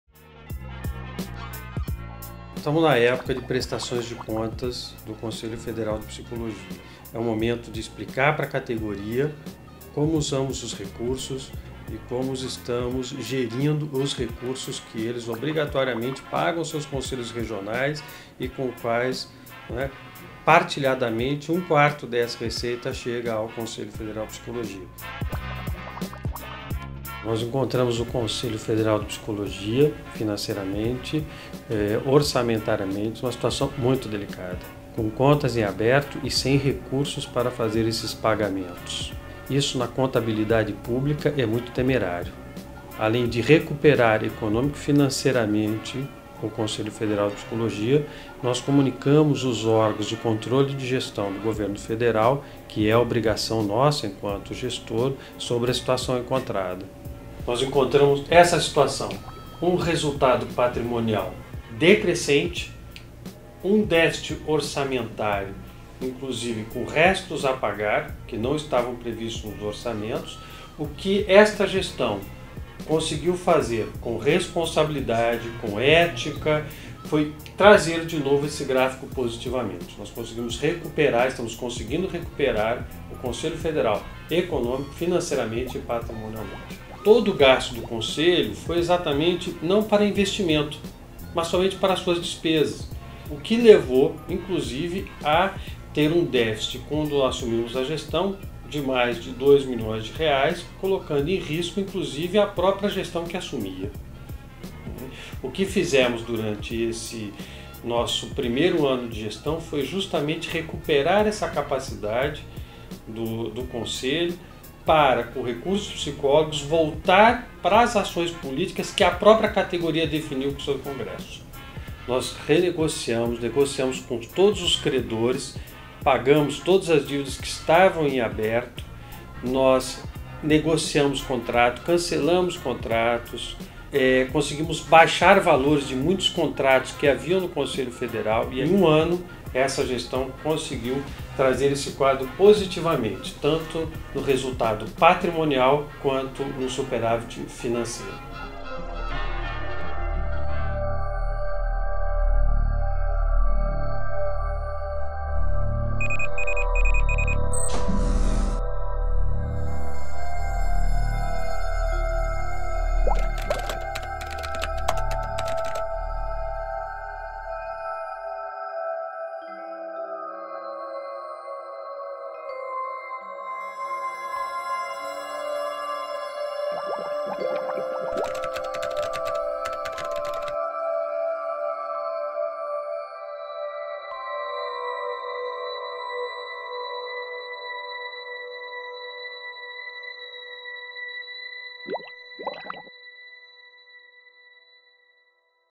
CFP reverte déficit orçamentário em superávit
A condução administrativa do XVI Plenário tem registrado eficácia na gestão financeira do Conselho Federal de Psicologia. Já nos primeiros meses de 2014, quando teve início a gestão, foram desenvolvidos cortes de desperdícios, revisão e renegociação de contratos e a racionalização do quadro de funcionários, além da contratação, em caráter inédito, de uma auditoria interna para avaliar e aprimorar os processos de gerenciamento de riscos, controle e governança. Leia mais